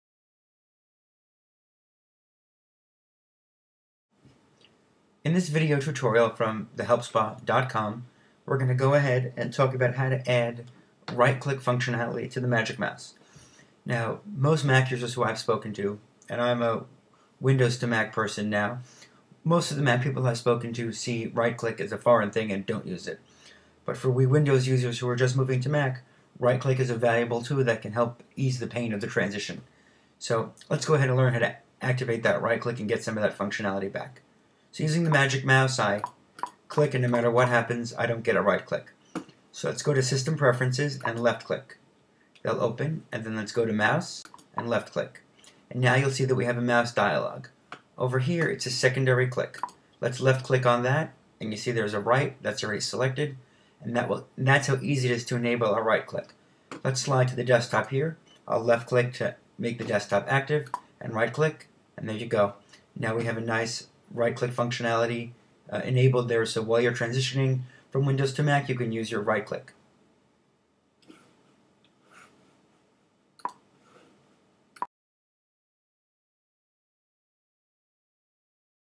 OS X: How to Enable Right-Click with the Magic Mouse
This tutorial from the HelpSpa will go over how to enable right-click in OS X on a Magic Mouse.